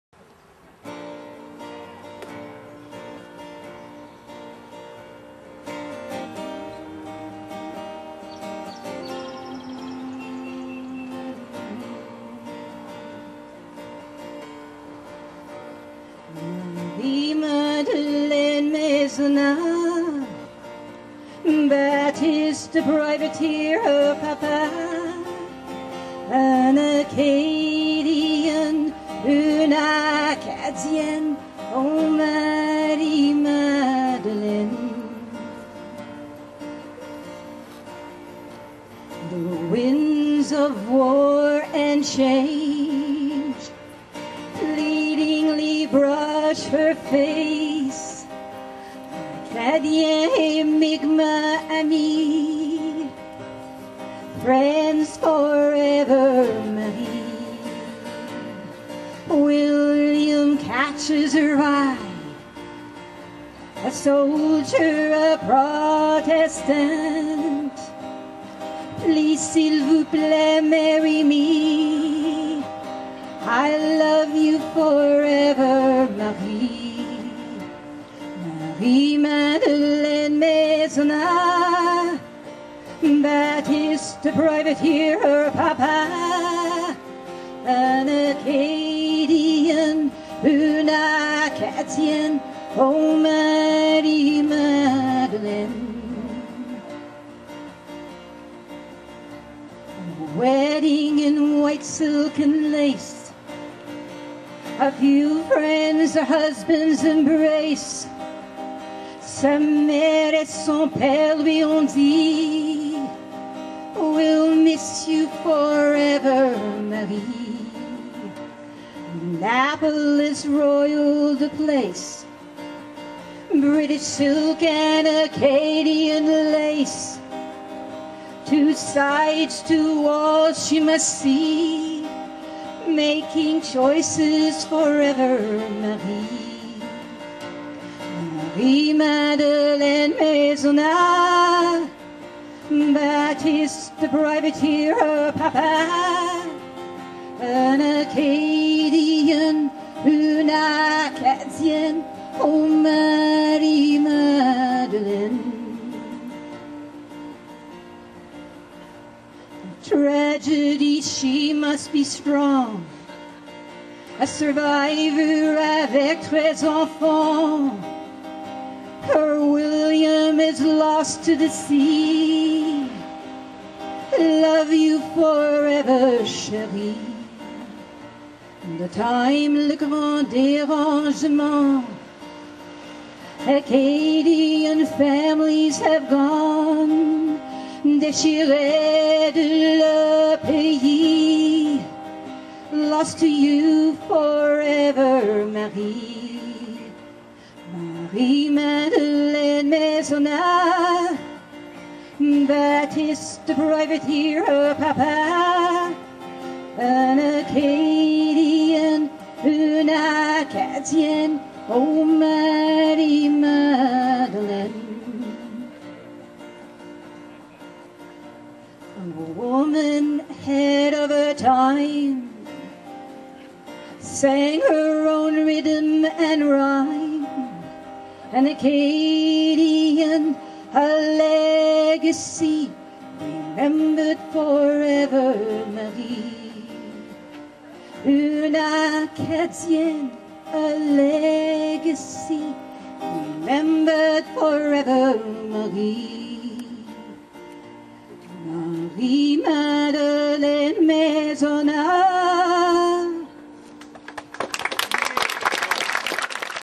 Jeanne Doucet Currie 2
Jeanne Doucet Currie sings at a Garden Party in Annapolis Royal, Nova Scotia, July 12, 2008.